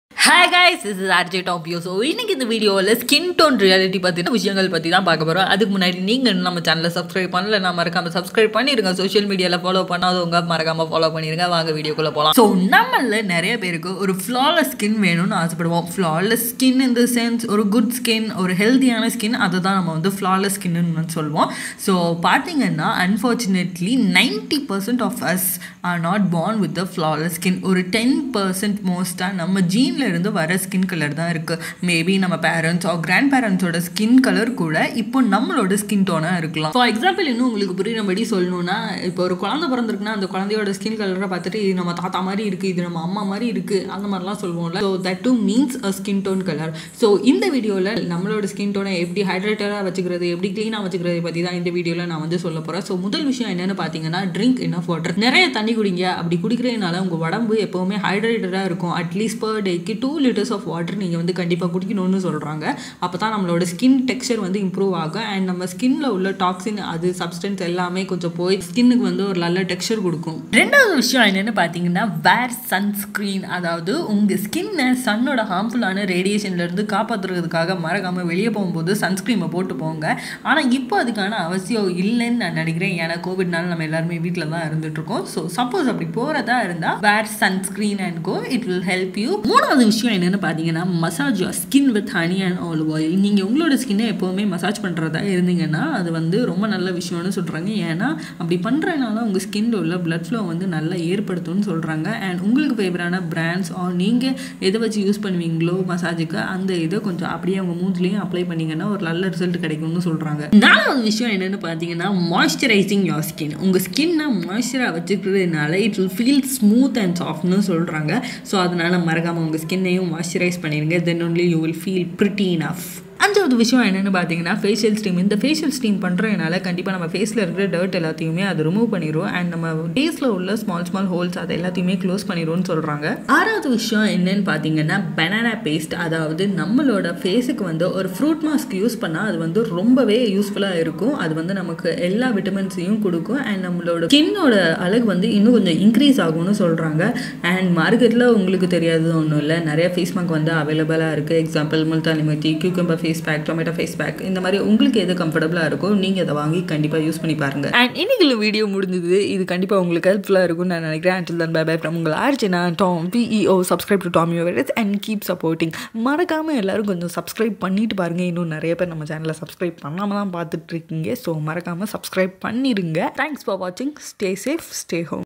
SkinTone Reality - Explained | tamil
Having a beautiful skin with an even tone and texture, is the need of every person. Especially women are more conscious about their skin and its well-being. For over the years, women remained to be fond of having a lighter skin tone. Nowadays, this has been added with a glowing and blemish free complexion. With the constant change in fashion sense, these things also keep on changing. Today, our busy lives want to achieve more while investing less time and money. It's all about being beautiful as well as intelligent.

For more updates do Follow tommy viruz

LETS BE FRIENDS ON SOCAIL MEDIA

VOICE OVER & EDITING : RJTOMPEO